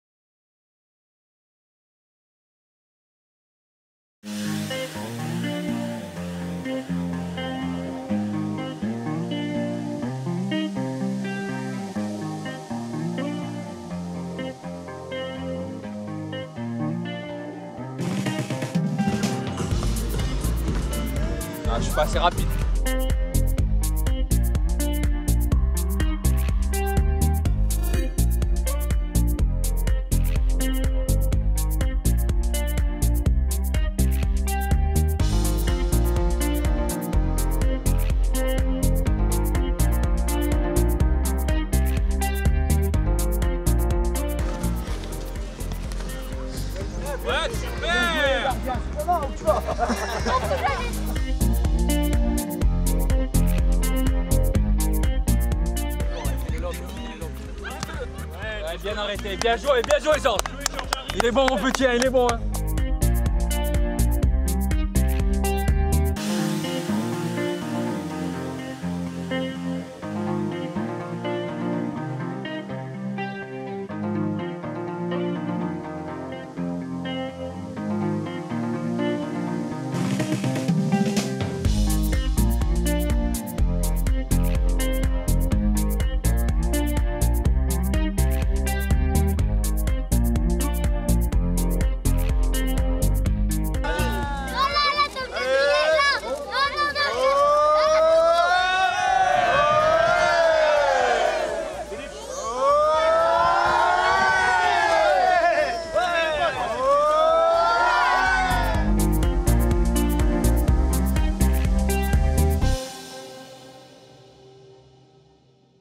Coach en nord : saison 3
Le club de Ruitz avait le plaisir d'accueillir ce mercredi après-midi, dans le cadre de Club en Nord, l'équipe professionnelle lensoise et son staff. Au programme : ateliers avec ballon animés par les joueurs et pour finir une séance de dédicaces. Retour en images...